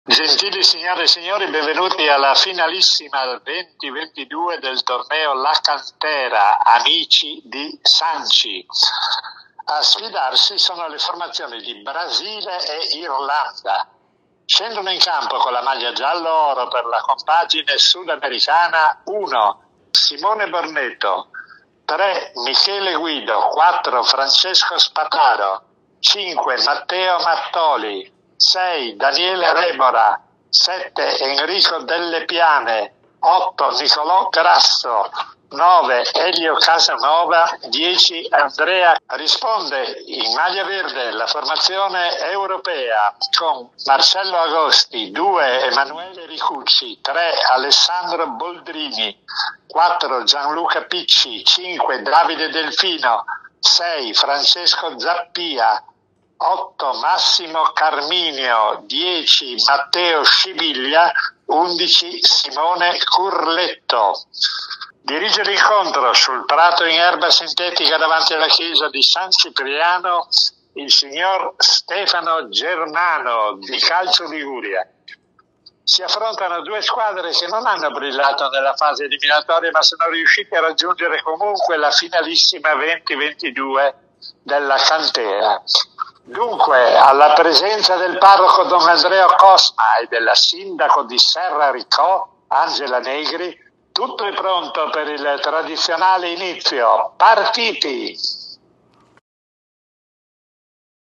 Cantera di San Cipriano: Bruno Pizzul legge le formazioni della finalissima
Vittoria ai rigori per il Brasile nella finalissima 2022 della ritrovata Cantera di San Cipriano: il torneo a iscrizione singola protagonista per oltre tre settimane sul colle di Serra Riccò.
Oltre alla consueta vitalità che anima il borgo, nella prima parte di giugno, con gioventù e stand gastronomici, chicca di assoluto prestigio nei minuti precedenti l’ultima partita del torneo.
Le formazioni e il prologo dell’atto finale, addirittura, a cura del telecronista Bruno Pizzul.
Pelle d’oca per gli organizzatori e gran parte del pubblico, cresciuto o invecchiato con quella voce fissa durante le telecronache della Nazionale.
Per i giovanissimi, il racconto di quanta storia sia racchiusa in quel contributo è stato a carico di genitori e nonni.
Nella lettura anche il vezzo da parte di Pizzul, come lui stesso usava ai tempi di Roberto Baggio, in riferimento a uno dei giocatori più talentuosi della squadra vincitrice, ossia, il numero 10 Andrea Masnata, citato soltanto in qualità di Andrea che dopo aver trascinato i suoi in semifinale ha dovuto dare forfait nella gara per il primo posto.
Omaggio speciale, infine, per il parroco e la sindaco che, sul palco, ha premiato da trionfatore il figlio Francesco Spataro da rugbista alla vittoria della Cantera grazie alla realizzazione del suo rigore decisivo.